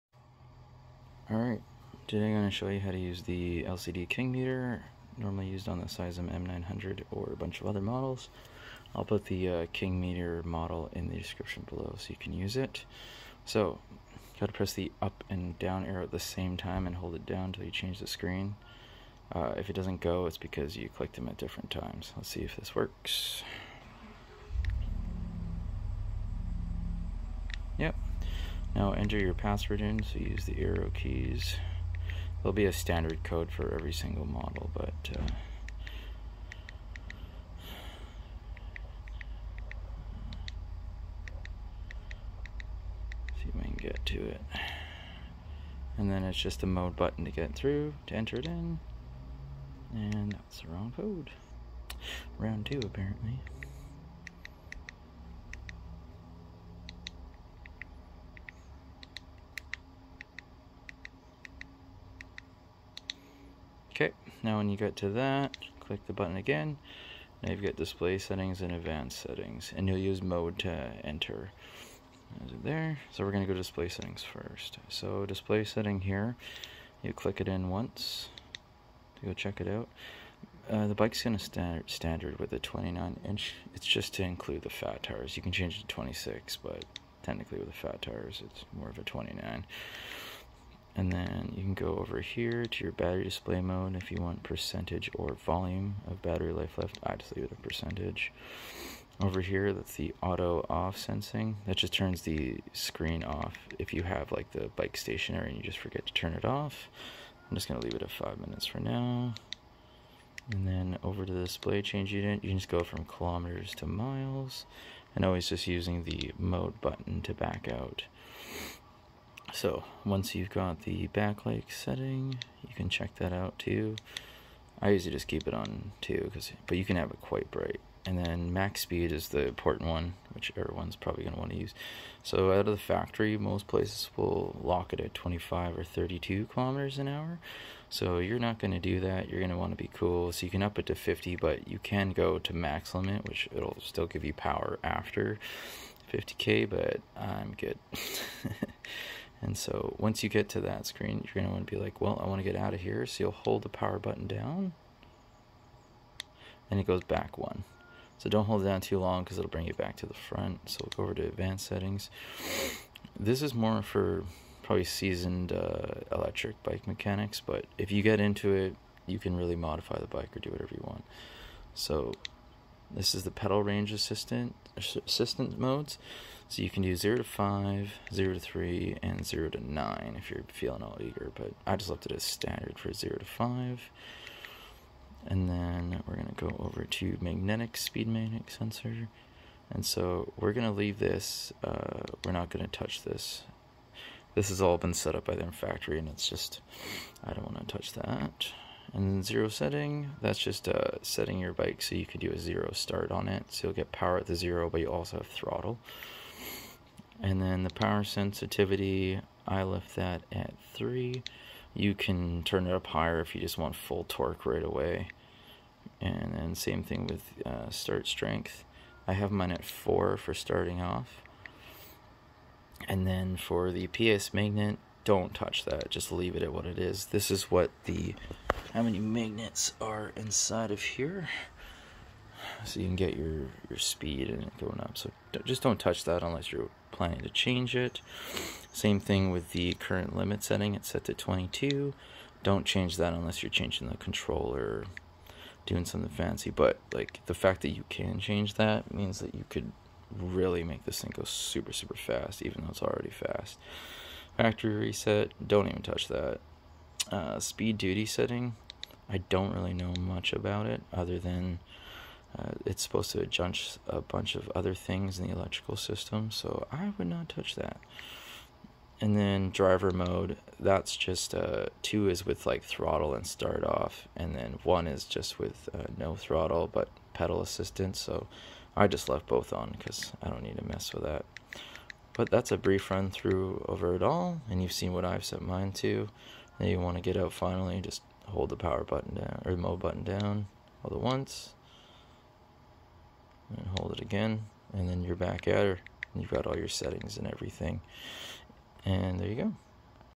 How To Use The King Meter LCD K5242
Showing how to use the king meter lcd on my cysum m900. Applies to other models as well.